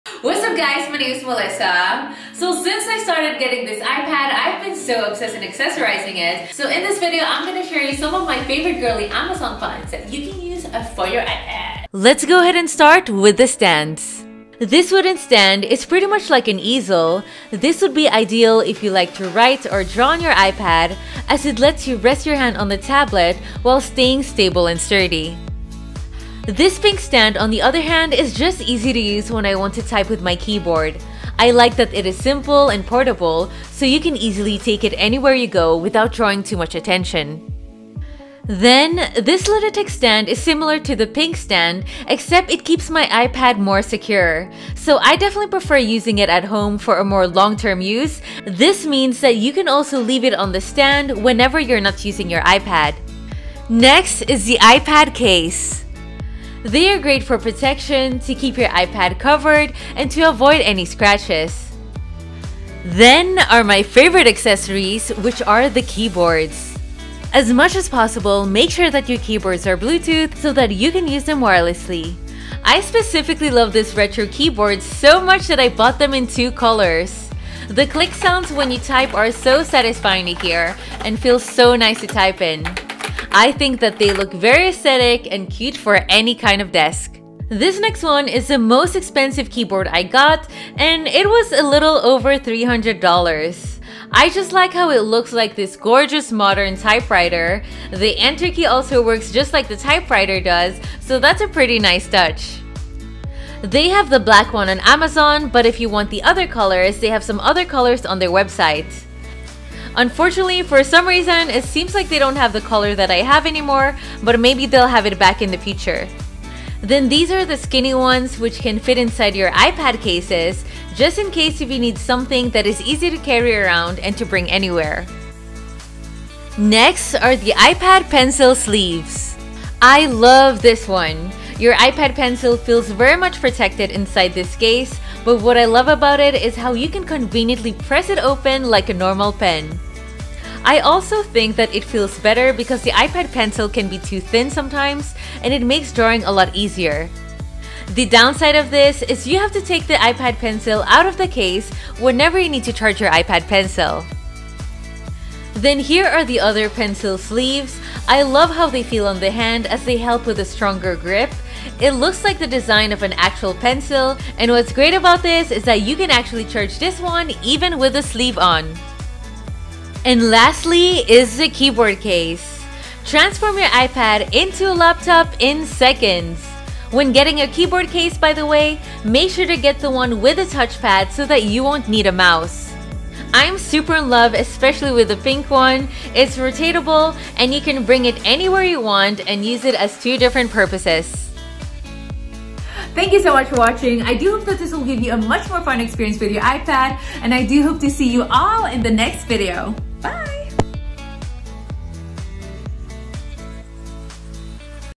Best Aesthetic iPad Accessories | Amazon Finds for Tablets - students, retro vintage keyboard
Here are some of the favorite aesthetic iPad accessories you can find on Amazon.
Whether you are a student or an artist with an iPad Pro, iPad air or iPad mini you'll find some cute ways to accessorize your tablet which includes a modern retro keyboard.

Here are all the links of the accessories from the video:

1. iPad Stands

Wooden Stand
Minimalist Stand
Ledetech Stand
Bigger Ledetech Stand

2. Tablet Cases
Pink Cover
Cosmo Case

3. iPad Keyboards
Brown Retro Keyboard
Pink Retro Keyboard

Pink Slim Keyboard
White Slim Keyboard

4. iPad Pencil Cases and Sleeves
Pen Case
Pink Pencil Sleeve

5. Keyboard Case

Hope this helped glamorize your iPad! Thank you all!